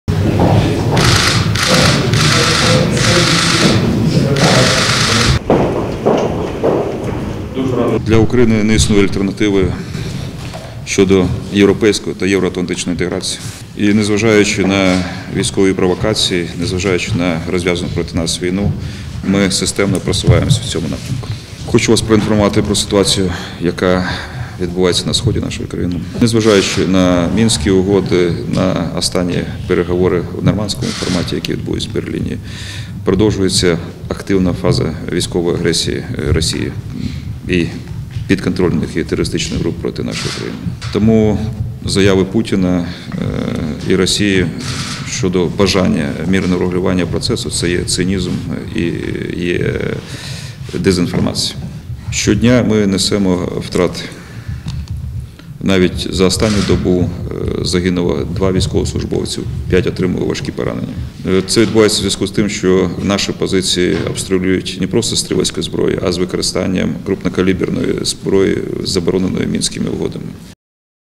Не можна припиняти санкції проти РФ, - вони тільки зараз почали давати результат, - Турчинов
Секретар Ради національної безпеки і оборони України, один із лідерів партії "Народний фронт" Олександр Турчинов провів зустріч з Головою Підкомітету з безпеки Європейського Парламенту Анною Ельжбетою Фотигою та депутатами Європарламенту.

Під час зустрічі співрозмовники обговорили ситуацію на сході України та агресію Росії проти нашої держави.

За словами О.Турчинова, Росія щоденно демонстративно порушує Мінські домовленості.

«Лише за поточну добу загинули двоє військових, п'ятеро зазнали поранень», - сказав він, додавши, що українські позиції обстрілюються із забороненого Мінськими домовленостями важкого озброєння: «Вогонь по наших позиціях здійснюється з 152-міліметрових гаубиць і 120-міліметрових мінометів тощо».

Секретар РНБО України наголосив, що поки не припиняться обстріли з боку російсько-терористичних угруповань, «будь-які спекуляції Росії навколо Мінська не мають жодного сенсу, а заяви Путіна щодо бажання мирного врегулювання - цинізм і дезінформація».

«Під прикриттям вогню здійснюються постійні спроби прорвати нашу лінію оборони і змінити конфігурацію лінії розмежування», - сказав він, додавши, що всі ці спроби одержують тверду відсіч з боку українських Збройних Сил.

Окрім того, О.Турчинов зазначив, що триває активна мілітаризація окупованого Криму: «Там розміщується наступальна зброя, в тому числі така, що здатна нести ядерний потенціал. Аналогічний сценарій розгортається на Балтиці в Калінінградському регіоні».

Секретар РНБО України додав, що дії Росії в Криму, нарощення потенціалу в Калінінградській області, створення потужних військових баз в Середземноморському басейні на території Сирії «свідчить про активну мілітаристську позицію Росії, яка не приховує своїх агресивних намірів».

За словами О.Турчинова, санкції, запроваджені кілька років тому, «тільки зараз починають давати результат»: «Саме тому зараз говорити про зменшення чи послаблення санкцій, означало б ламати той єдиний фронт, який склався в Європі проти агресора – Росії».

«Навпаки, - додав він, - слід почати посилювати санкції до досягнення конкретного результату - звільнення окупованих територій, припинення агресивних військових операцій РФ, і припинення гонки озброєння, яку зараз веде Росія».

Підсумовуючи, О.Турчинов наголосив, що для України не існує альтернативи європейській і євроатлантичній інтеграції:  «Не зважаючи на продовження агресії проти нашої держави та економічні труднощі, ми продовжуємо невпинно рухатись у цьому напрямку».

У свою чергу, А.Е.Фотига наголосила на підтримці Європейським Союзом реформ та євроінтеграційного курсу України та розумінні необхідності продовження санкцій проти РФ.

Європейські депутати висловили сподівання, що «лібералізація візового режиму з Європою для України відбудеться вже найближчим часом», та на ратифікацію Угоди про асоціацію всіма країнами-членами ЄС.

Приєднуйся!